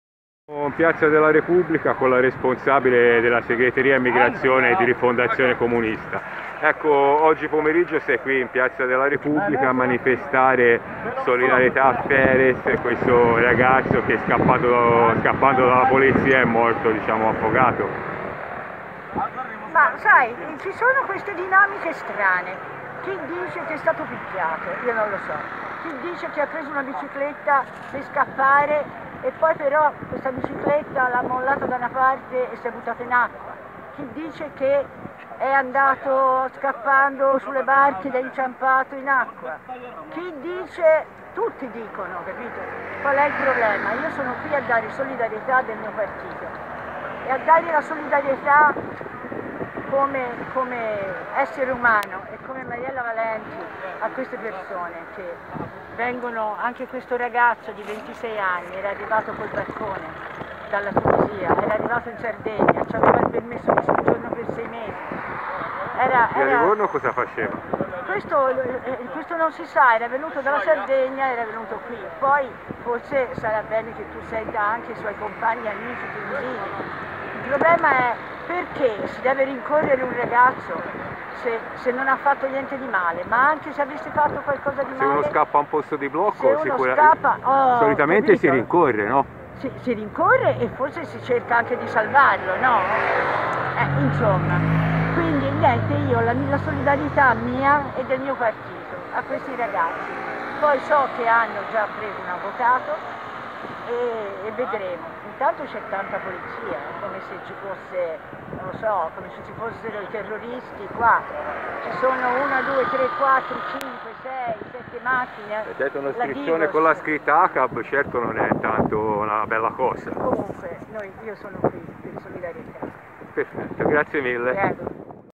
Tunisino affogaro, Rifondazione: "Ci sono strane dinamiche c'è chi dice sia stato picchiato"
Livorno 26 aprile 2021